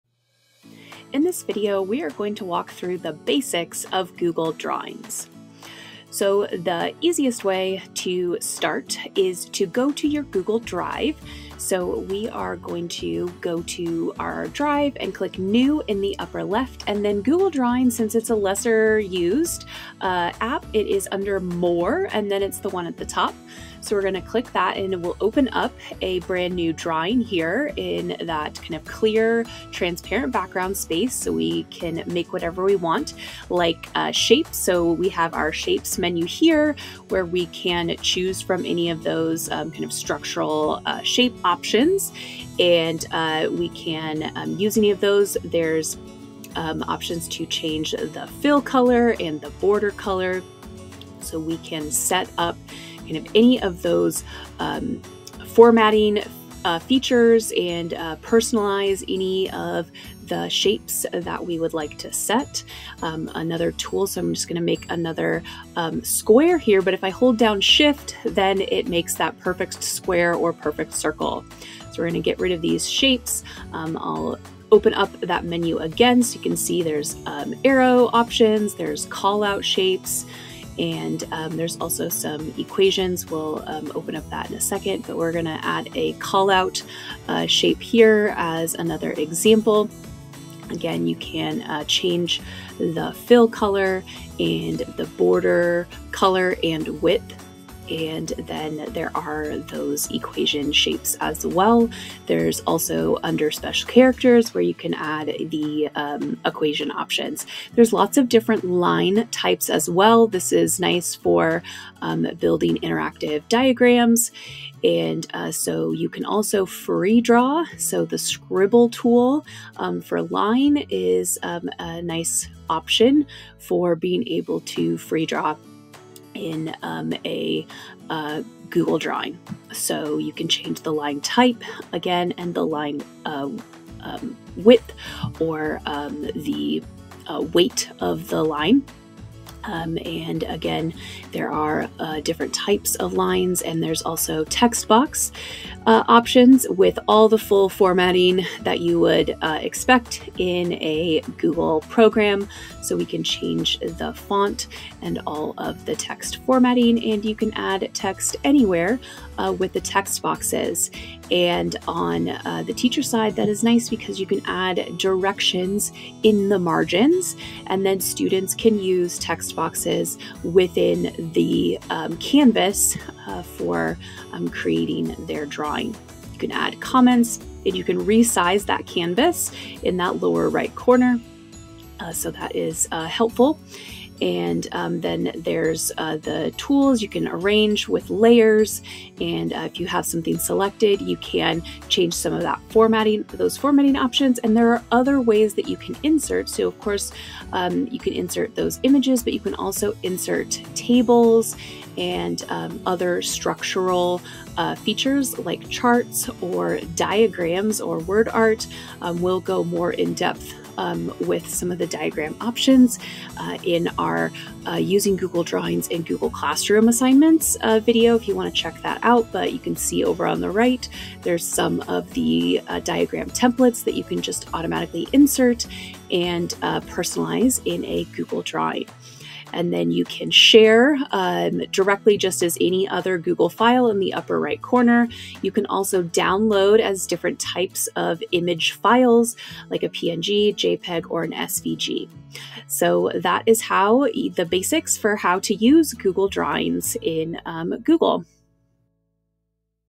How to Use Google Drawings
In this video we walk through the basics of how to use Google Drawings.